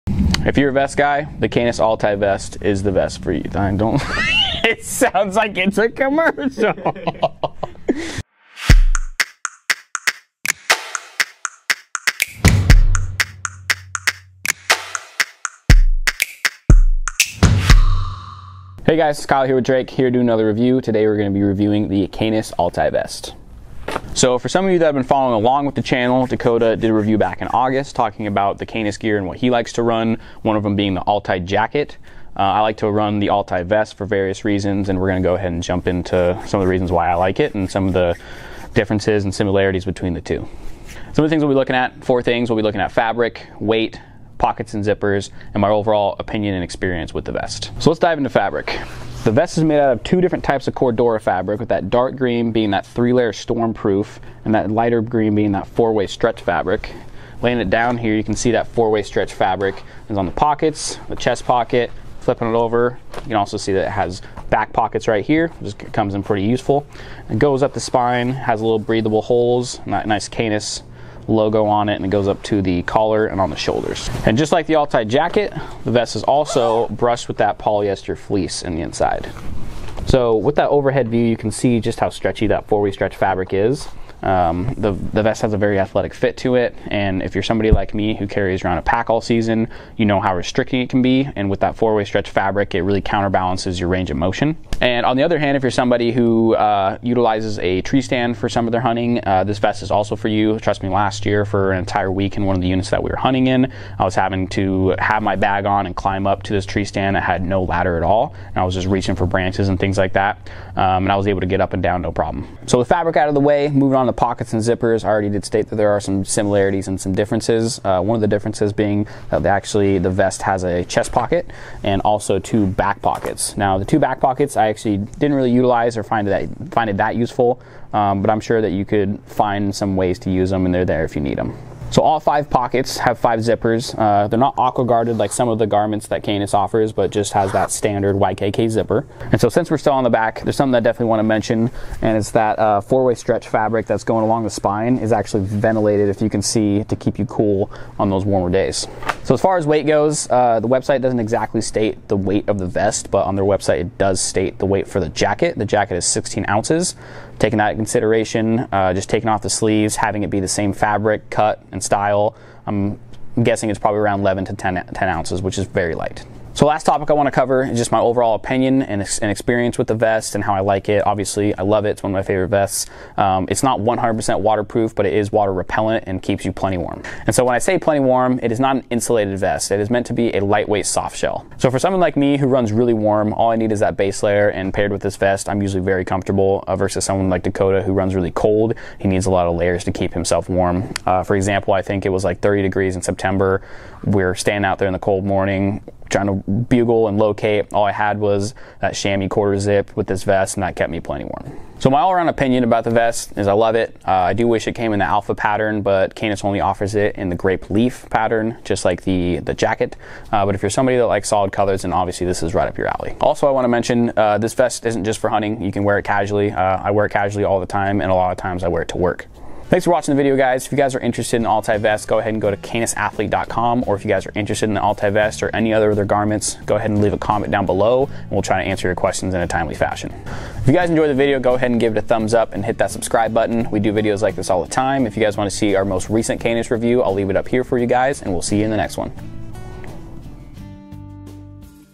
CANIS Altai Vest | In Depth Review
Welcome to another gear review. Today we cover the CANIS Altai Vest, the little brother to the Altai Jacket. We were fortunate enough to test out this vest during the archery season and it was a game changer.